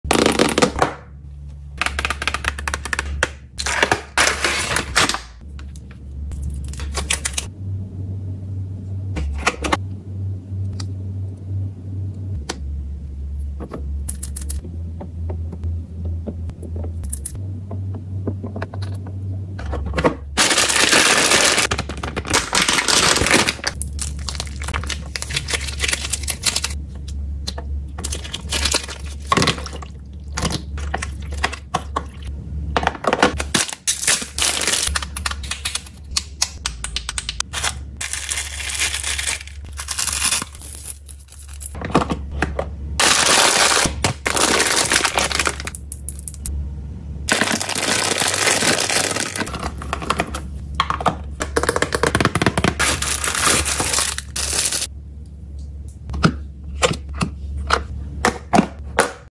Packing School Lunch with CANDY Food Satisfying Video ASMR!
Let's Pack this Lunch with a Bunch Sweet Candy Food!